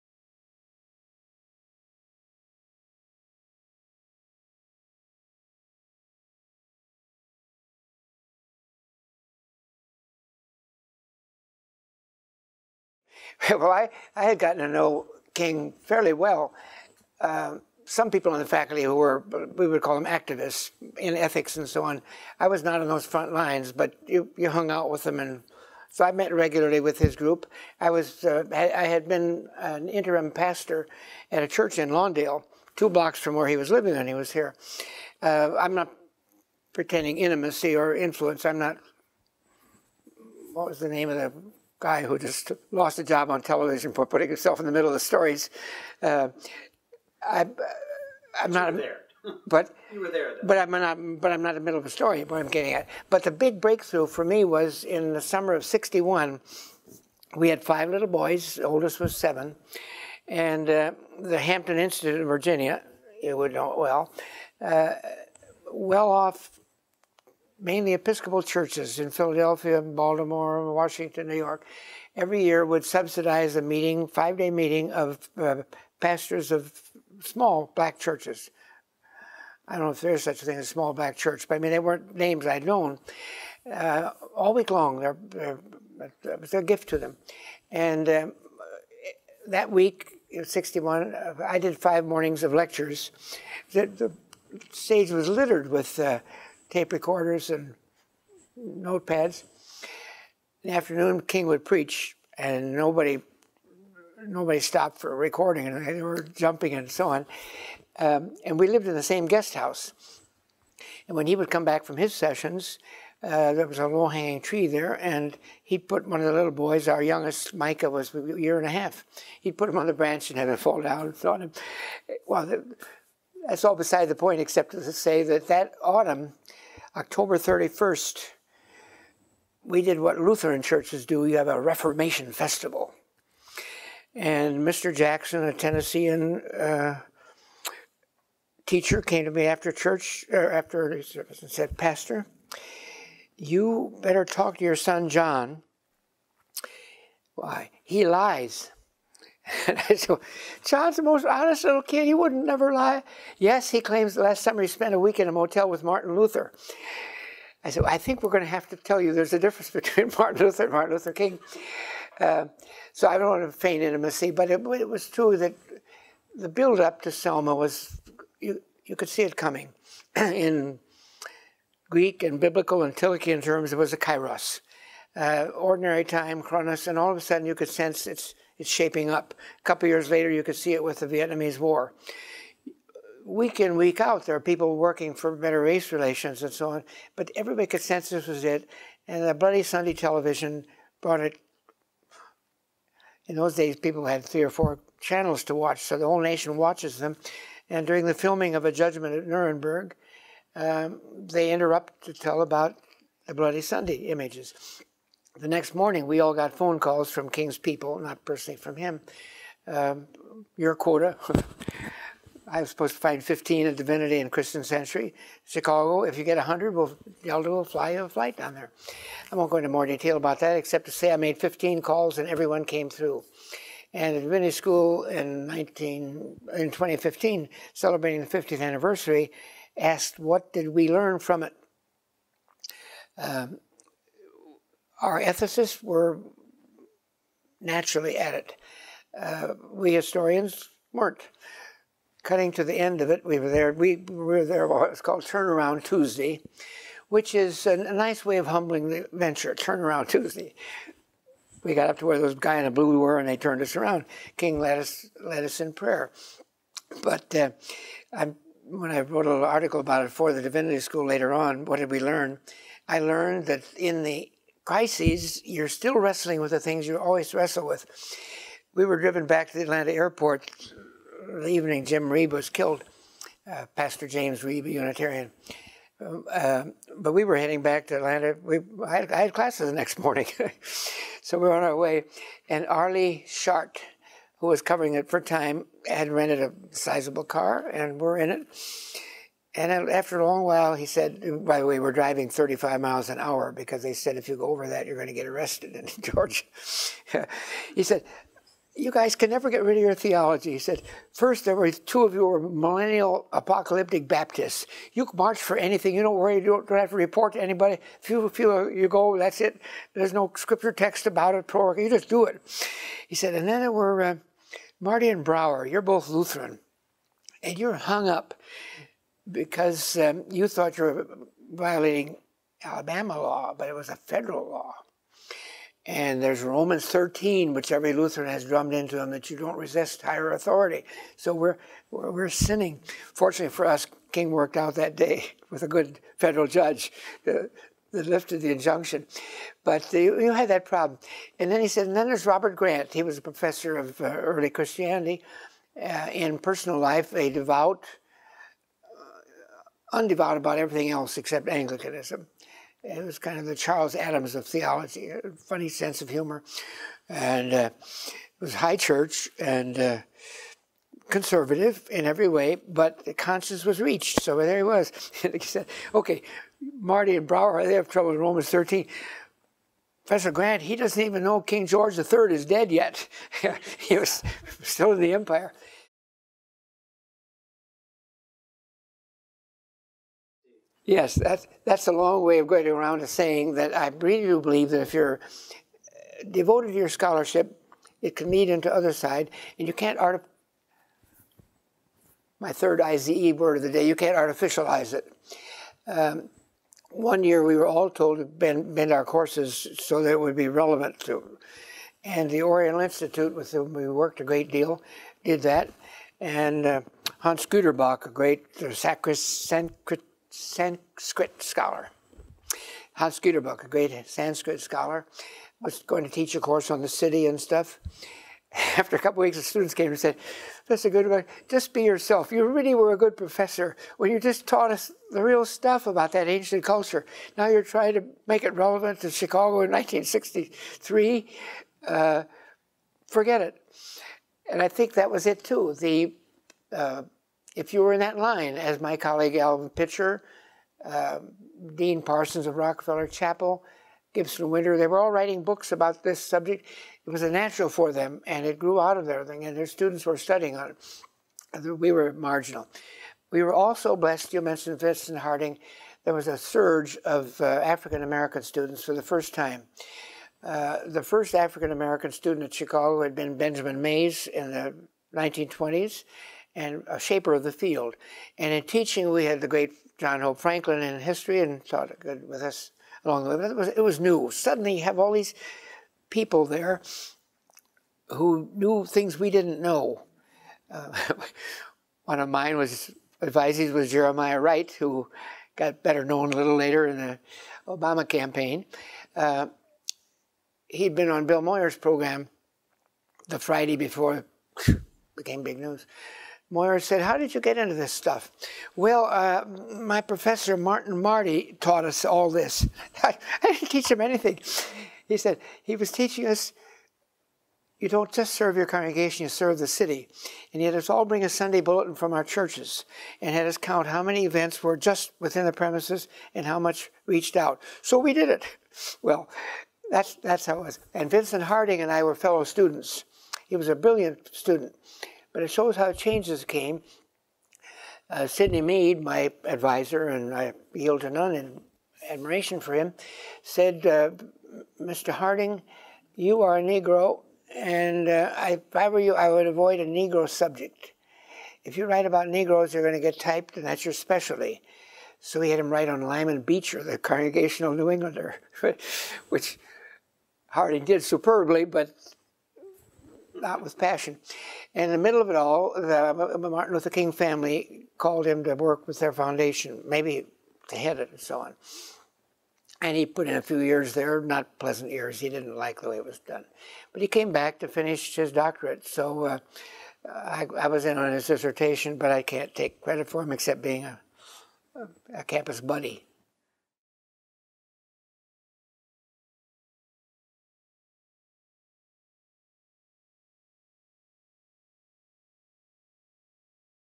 Martin Marty on Martin Luther King, Jr. and the Civil Rights
The civil rights movement of the 1960s included many religious leaders who strove to end segregation and discrimination in the United States. In this interview with Robert Wilson-Black, CEO of Sojourners, Professor Martin Marty discusses "Bloody Sunday" in Selma, Alabama, his work in the civil rights movement, the conflict between Lutheran teachings and civil disobedience, and his relationship with Dr. King.